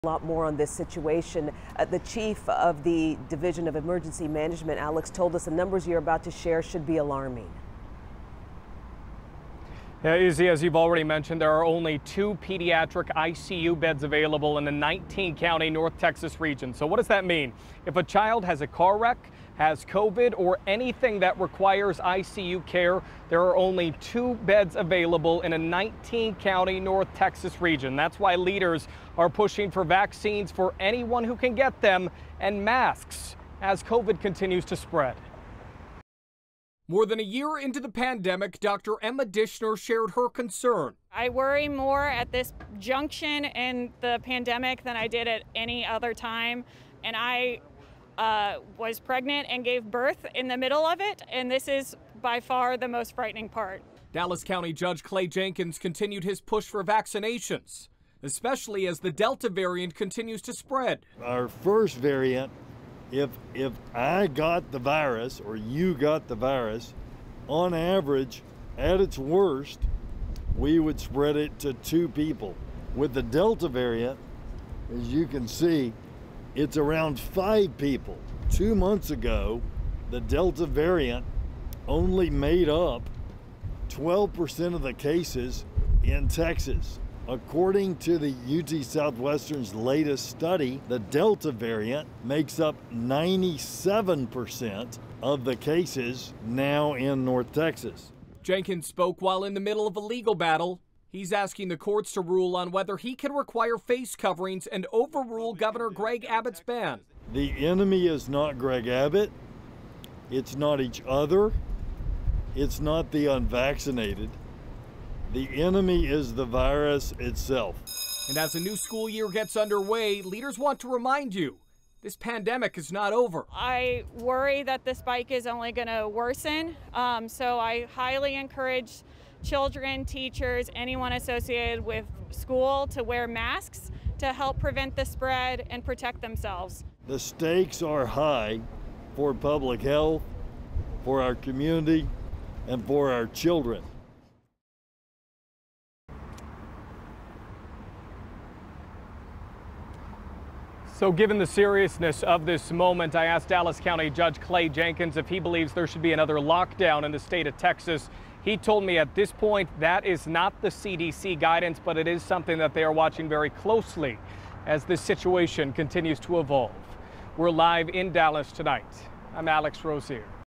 2 pediatric ICU beds available in 19 counties in North Texas
More healthcare leaders say this could be the most frightening part of the pandemic for North Texas.